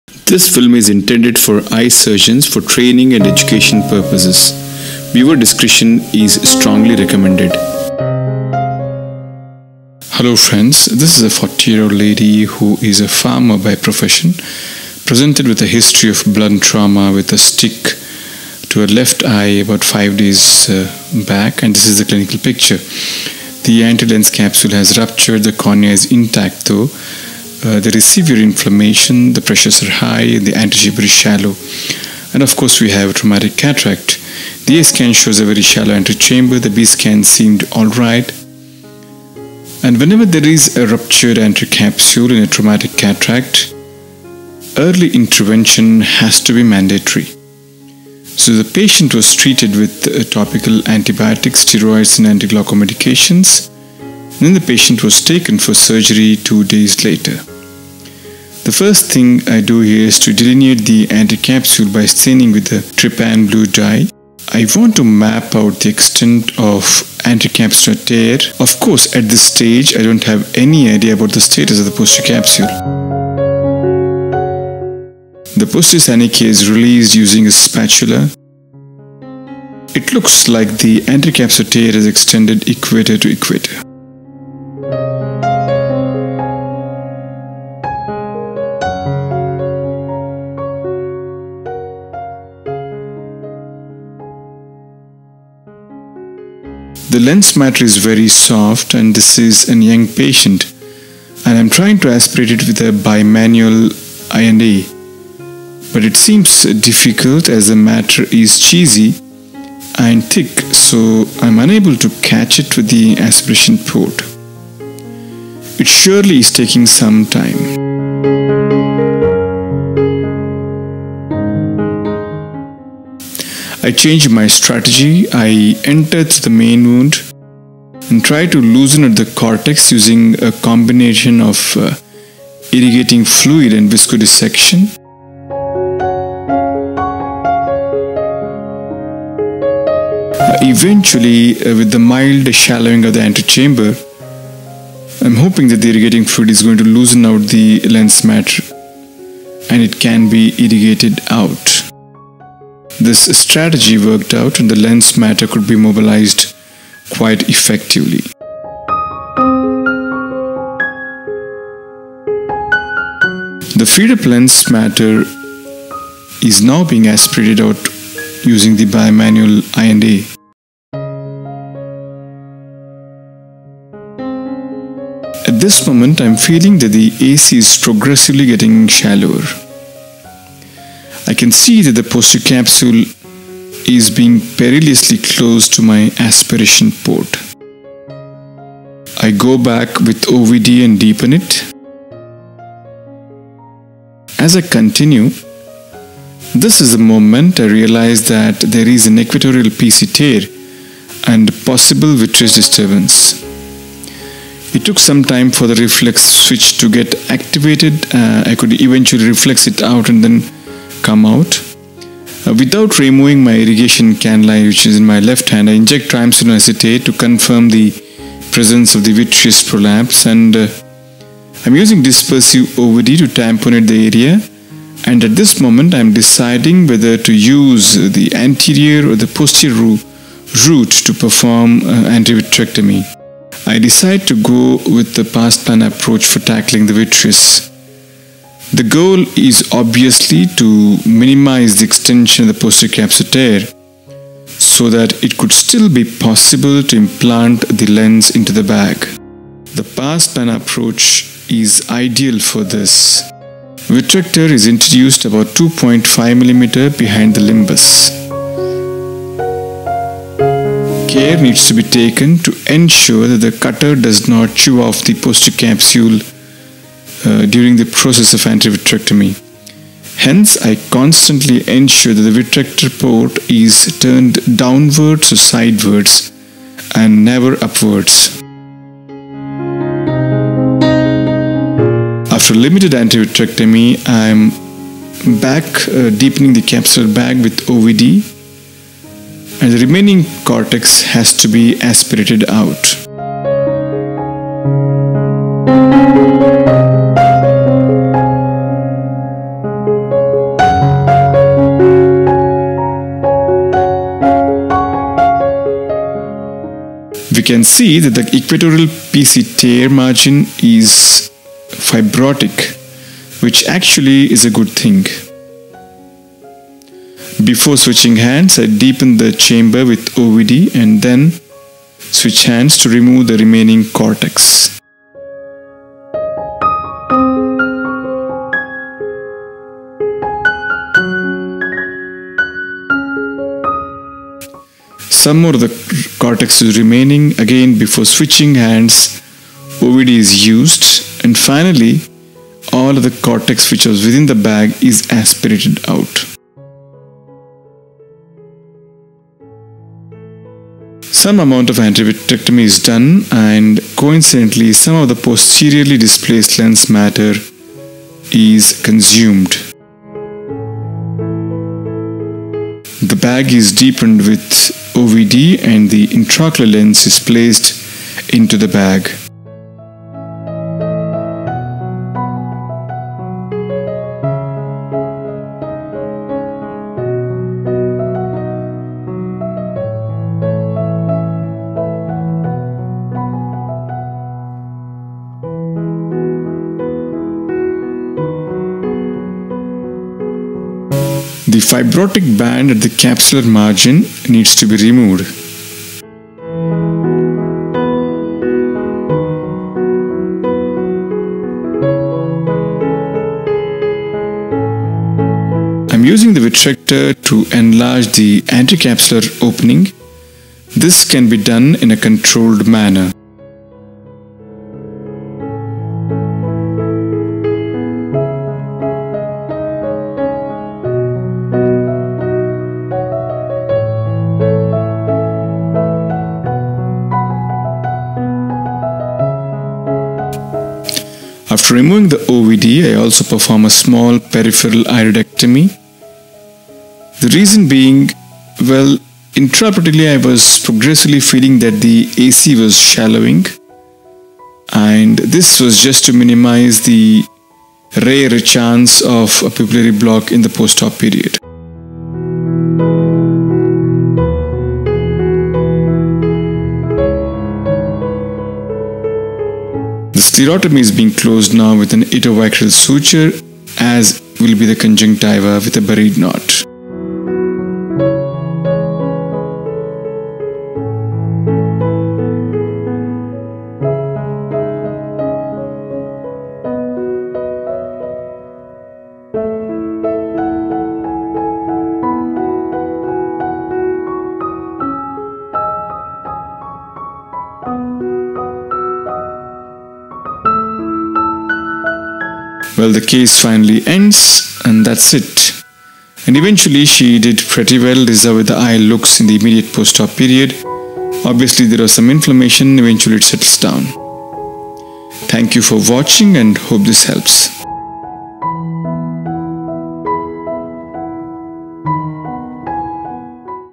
Traumatic Cataract With Pre existing PC tear - Dr Deepak Megur
This video is about a 40 year old lady wit traumatic cataract with rupture anterior lens capsule and secondary glaucoma. During the surgery it was found out that there was a pre existing equatorial PC tear as well . This video demonstrates the technique of using the posterior approach i.e Pars Plana approach  for anterior vitrectomy. After anterior vitrectomy and a cortical clean up , IOL was placed into the bag  and the case was completed. The Patient did have excellent visual acuity in the post operative period.